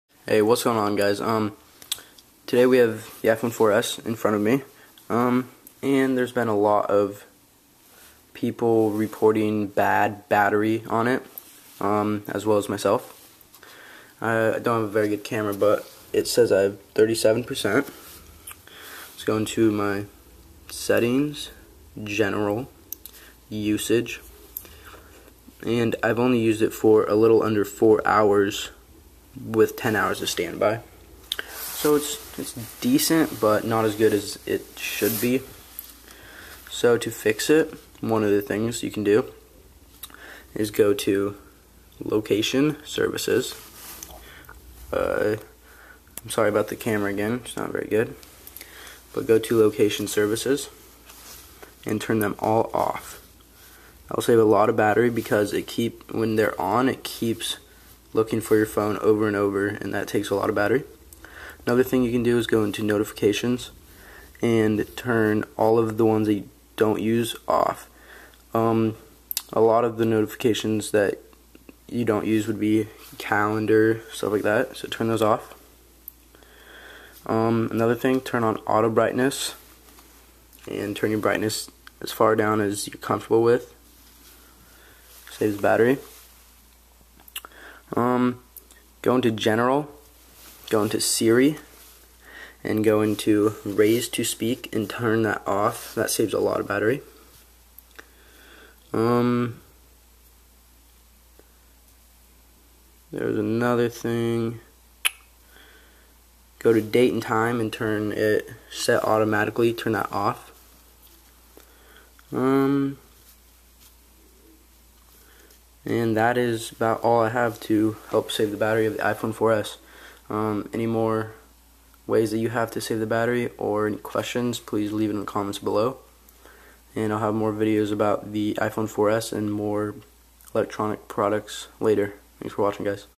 iphone 4S battery life problem FIX
a video of how to get more battery life on the iphone 4s.